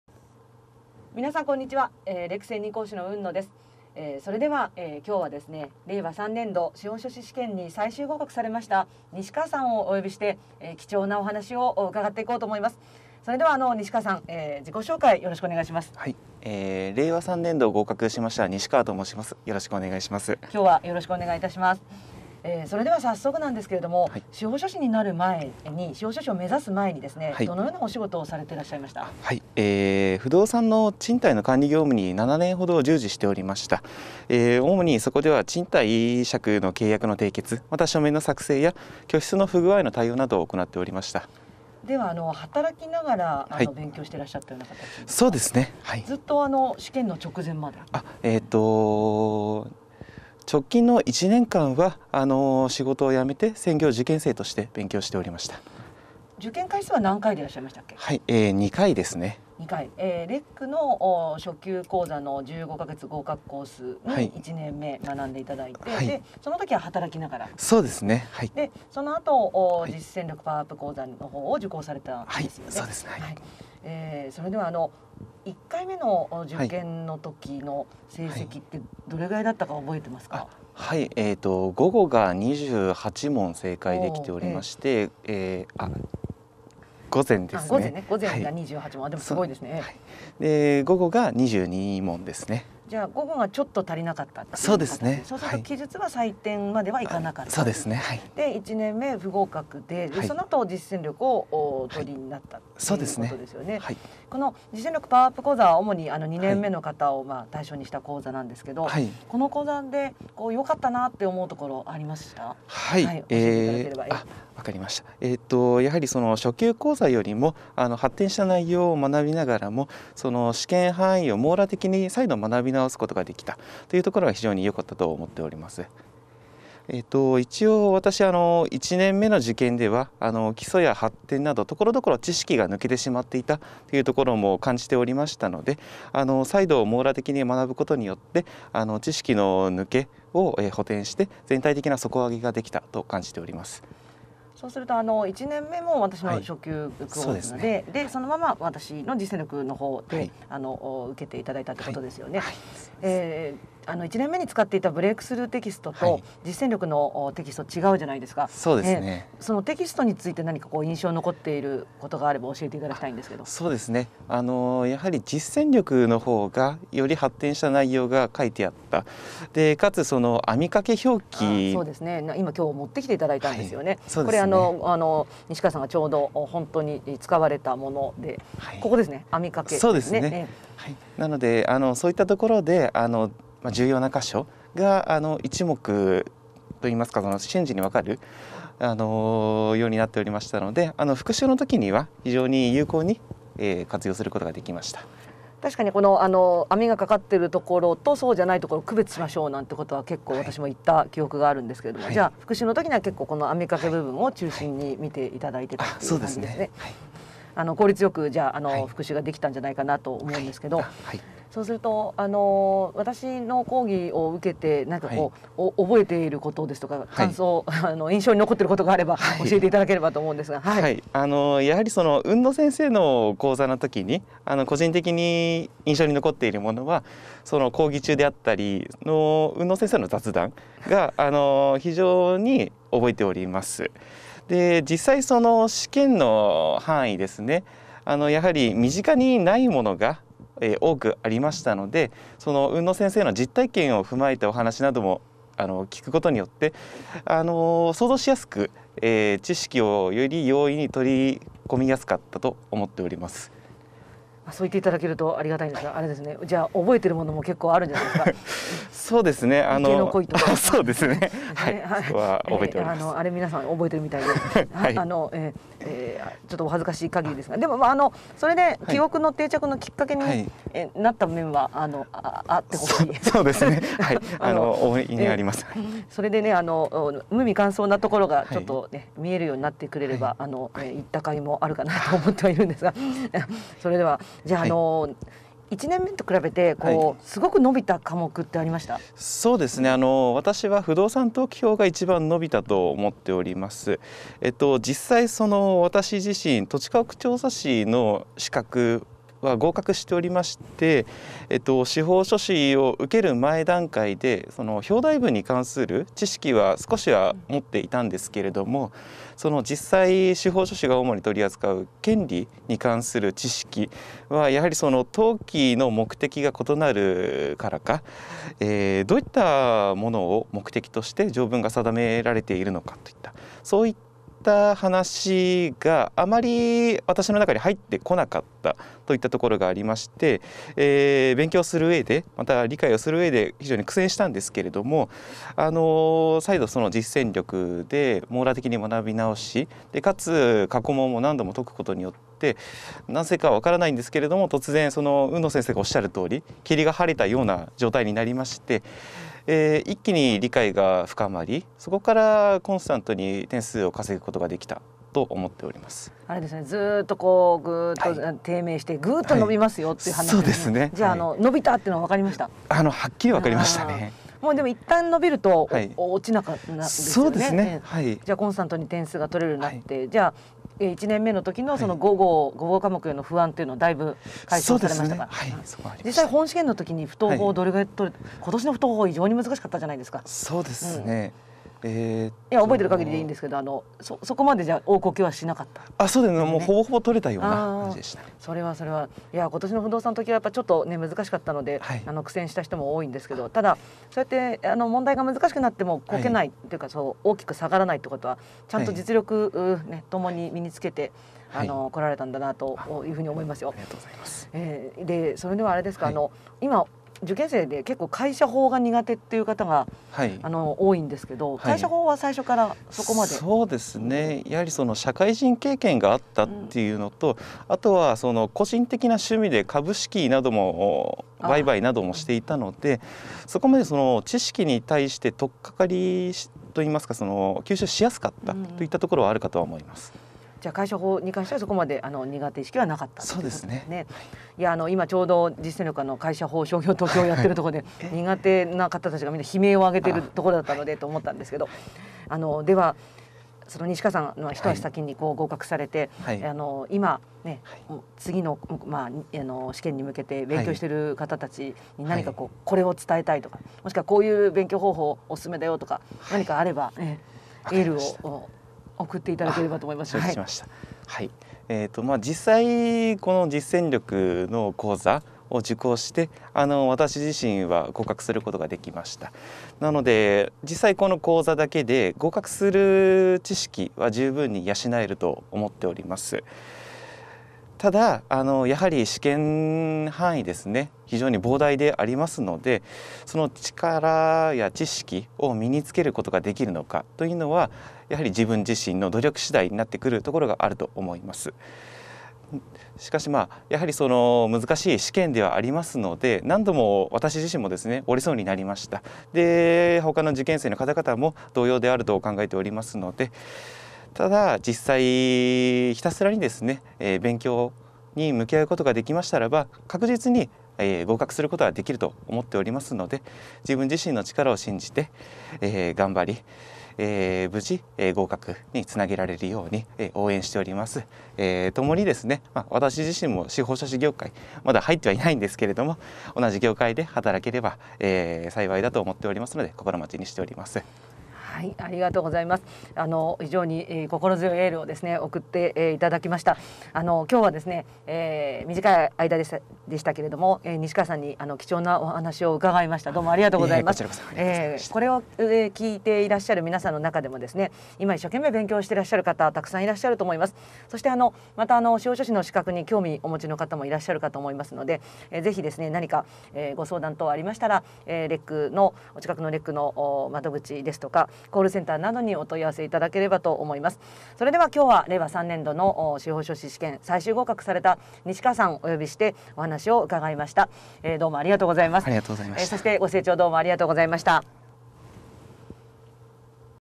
【司法書士試験】令和3年度合格者インタビュー LECで初級・2年目講座を受講して合格！
令和3年度合格者の西川さんはLECで初級講座を受講後、1回目の試験では午後の部で敗退。その後実践力PowerUp講座を受講して見事合格されました。
本動画では西川さんの実践力PowerUp講座の活用法を伺いました。

▼海野クラス生講義実施校／新宿エルタワー本校
随時受講相談承っております！